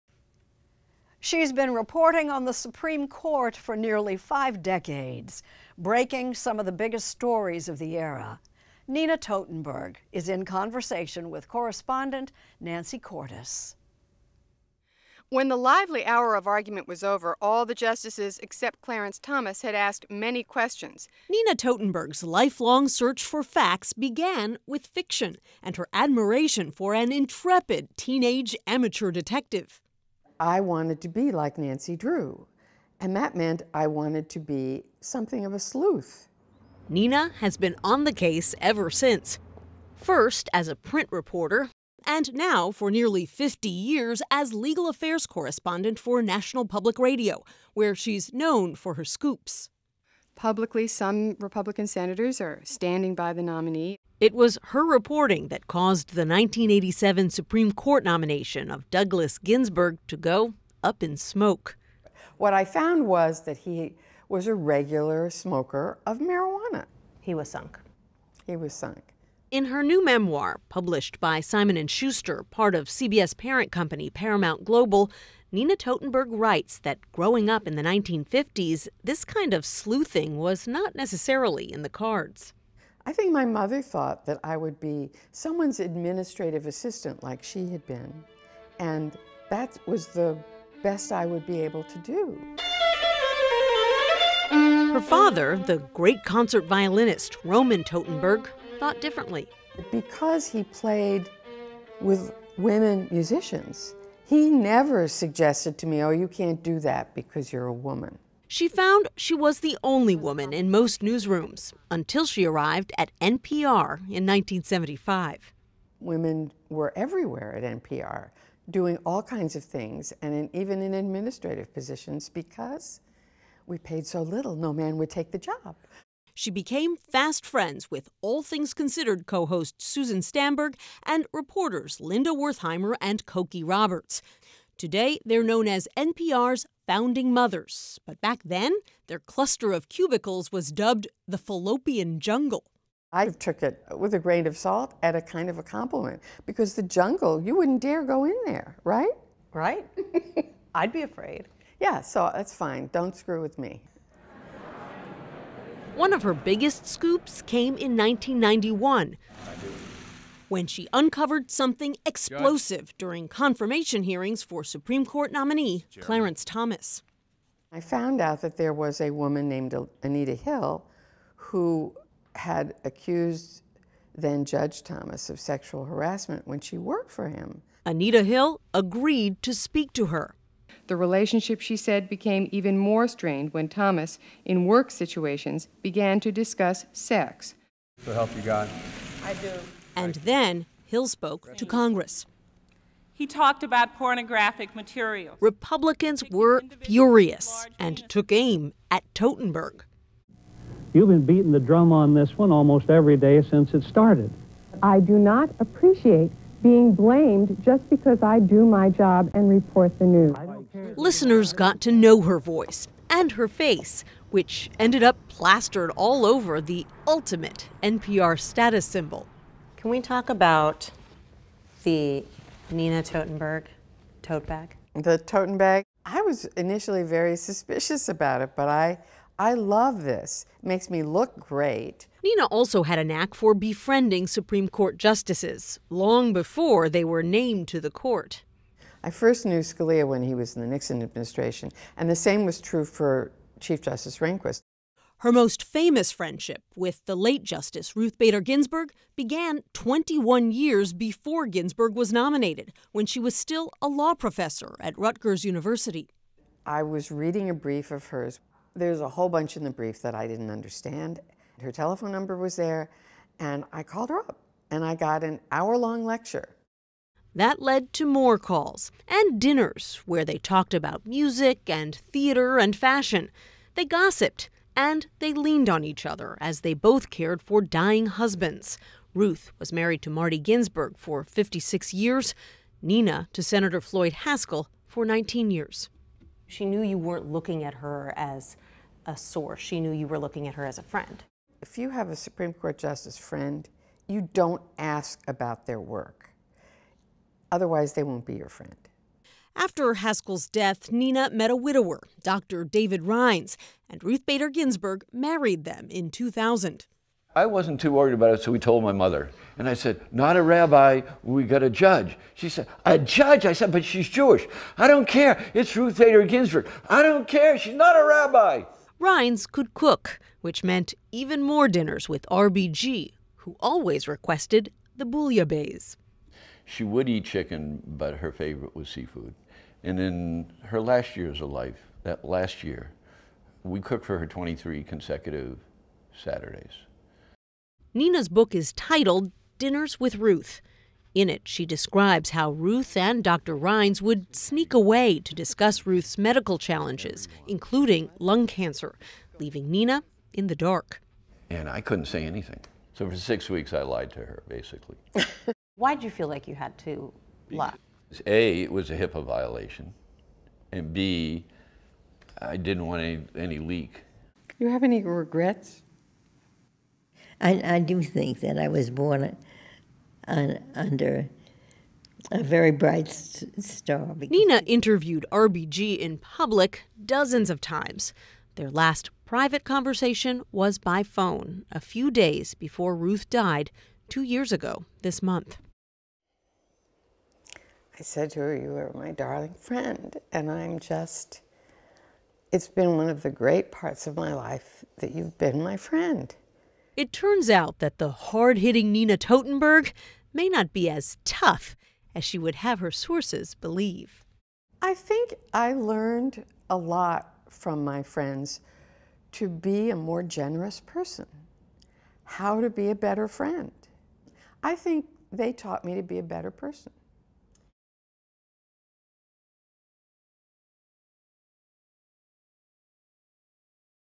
Nina Totenberg and her friendship with RBG
NPR's legal affairs correspondent Nina Totenberg is known for her insightful coverage, and frequent scoops, concerning the Supreme Court. CBS News chief White House correspondent Nancy Cordes talks with Totenberg about her new memoir, "Dinners With Ruth," and what she gained from her decades-long friendship with Justice Ruth Bader Ginsburg.